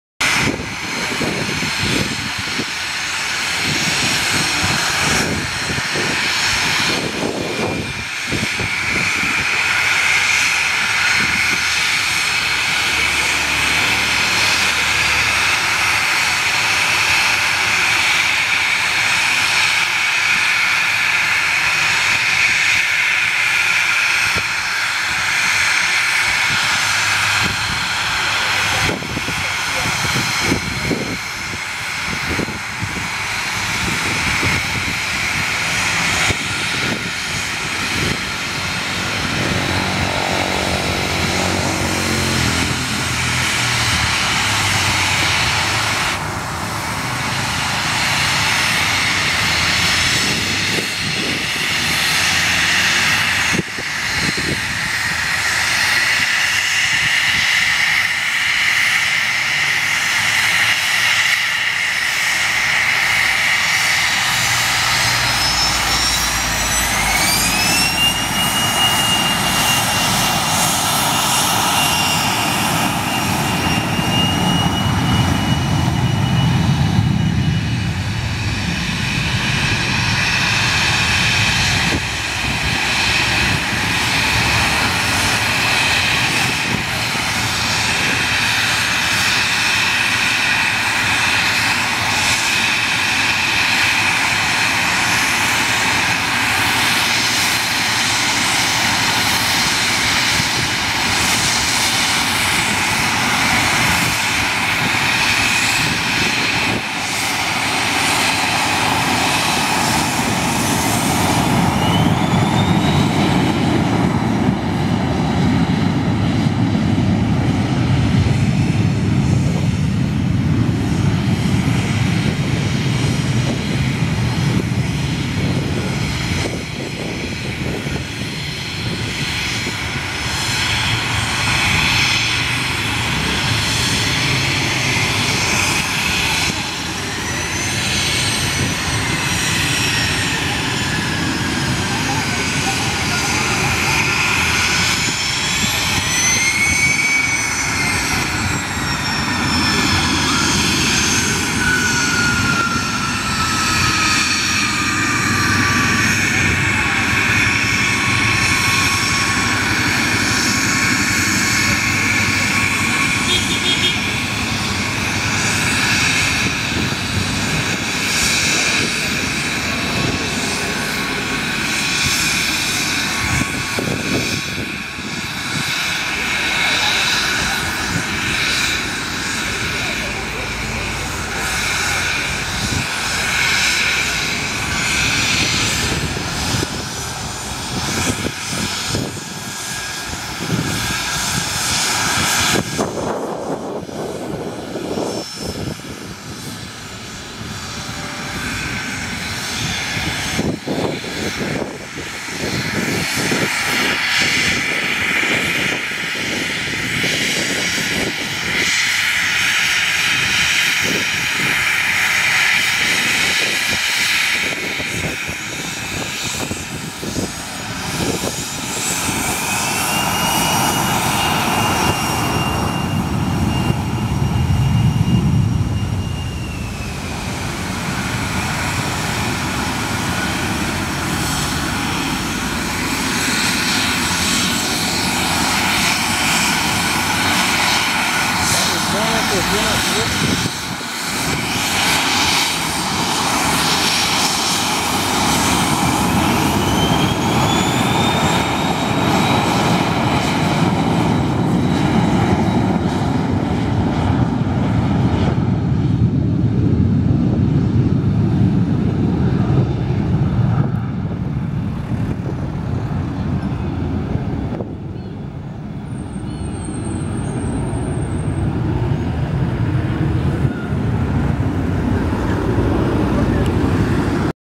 Embraer Phenom 300 2 Decolagens em Angra dos Reis RJ
Embraer EMB-505 Phenom 300 PR-NGM & PR-GCR Decolando de SDAG